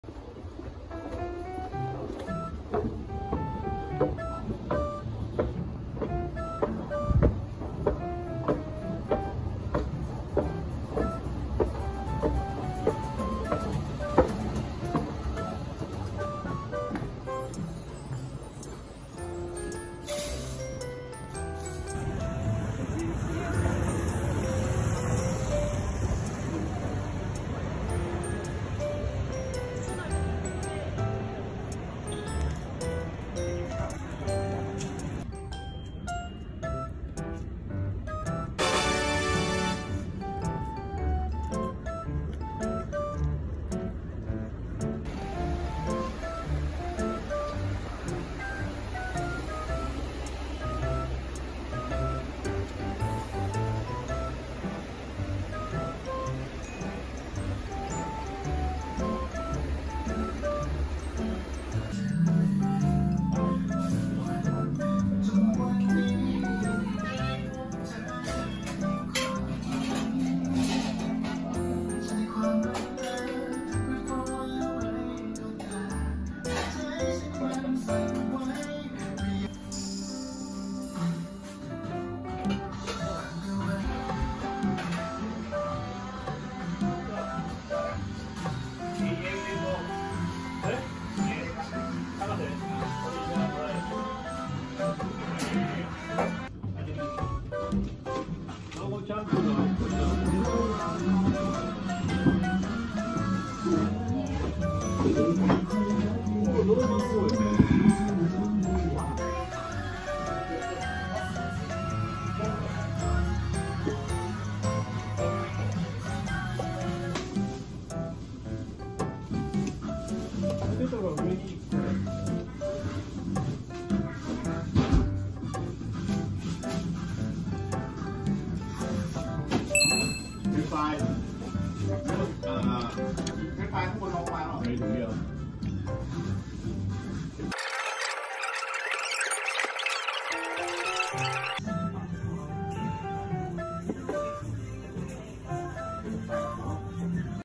福岡 iPhone15購入後、大名にあるガムランディでランチ
アラフィフおやじが福岡にあるタイ料理ガムランディでガパオライスのランチを食べた動画です